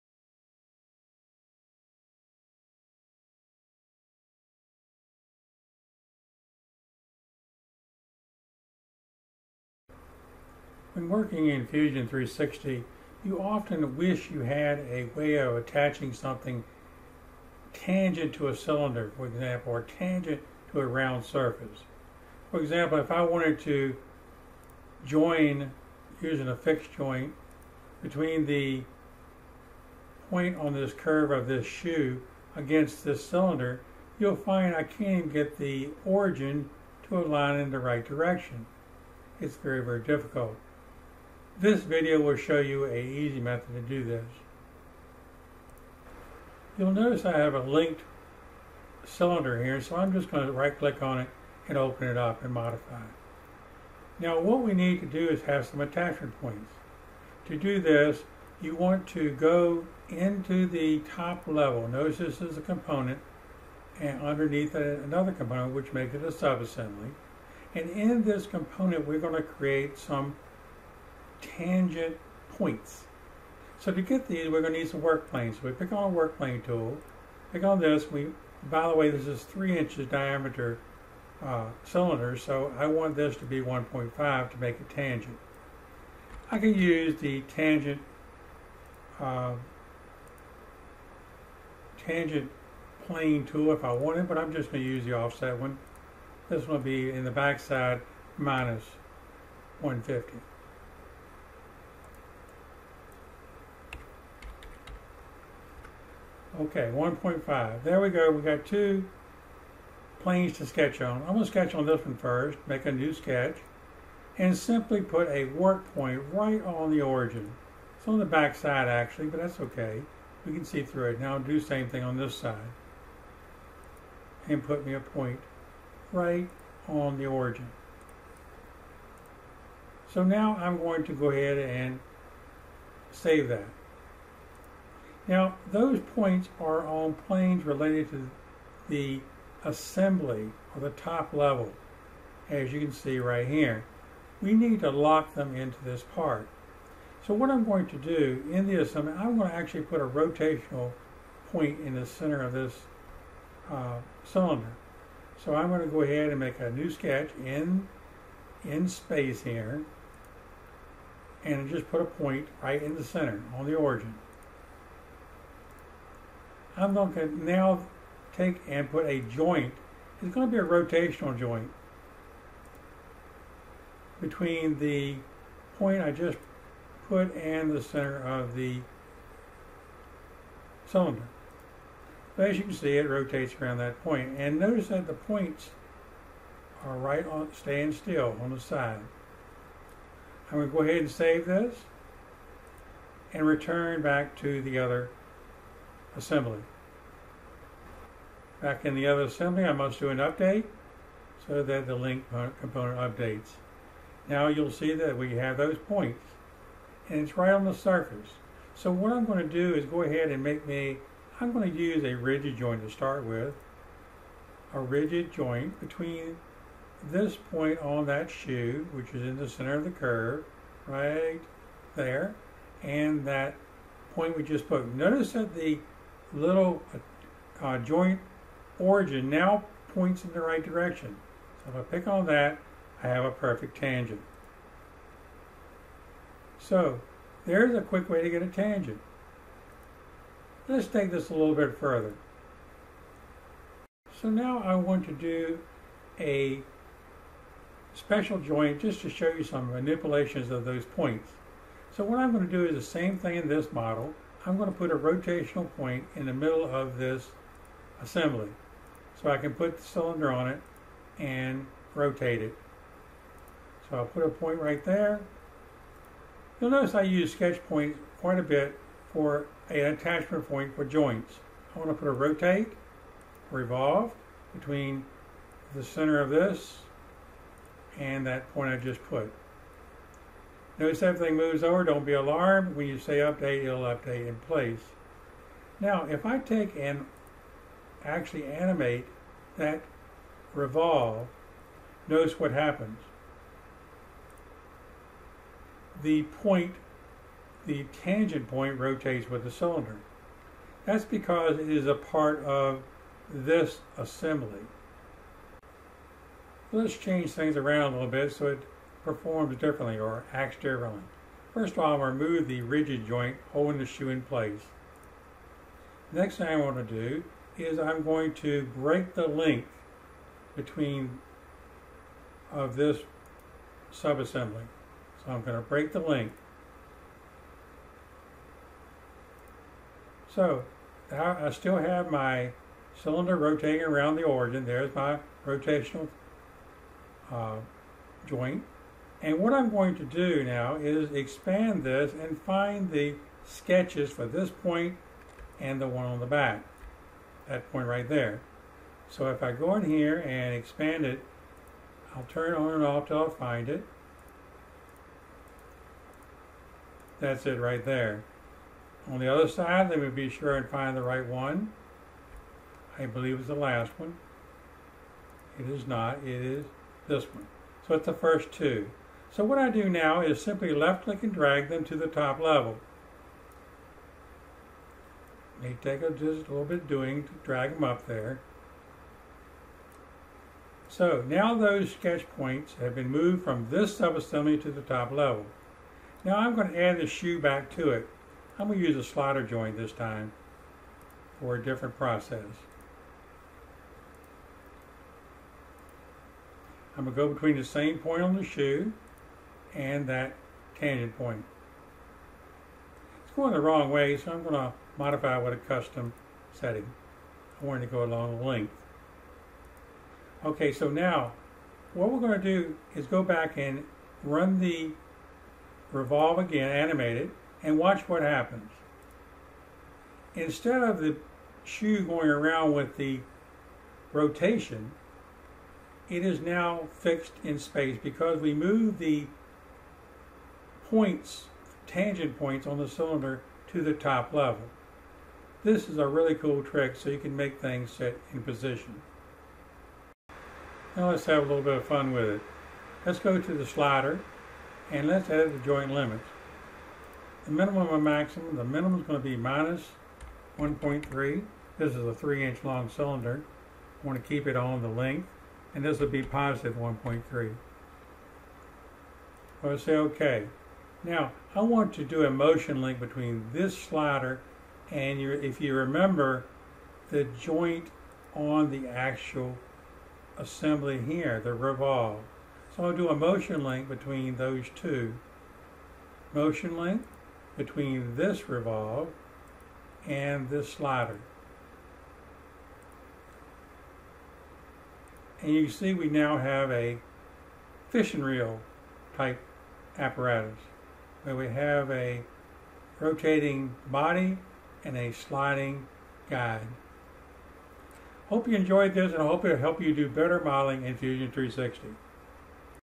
Creating Tangent Joints in Fusion 360
How many times have you wished for a tangent joint in Fusion 360?  If so, this video is for you.  I also try to explain how to get special motions from these tangent joints.